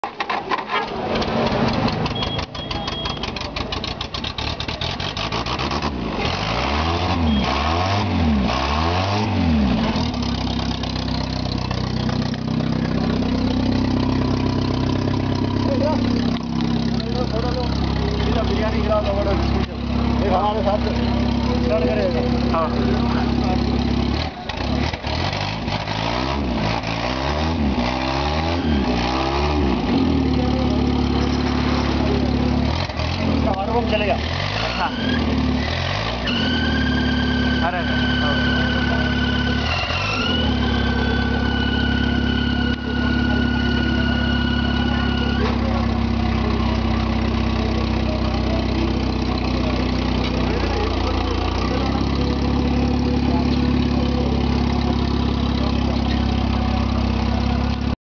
My 1960 Standard Pennant (now SOLD) firing up the 1st time!
My 1960 Standard 10/Pennant just rescued from the scrappers being cleaned & started up at the breakers'. The car appears to have not run since a decade! Engine turns- just ! Water-pump gone though, as my mech. points out.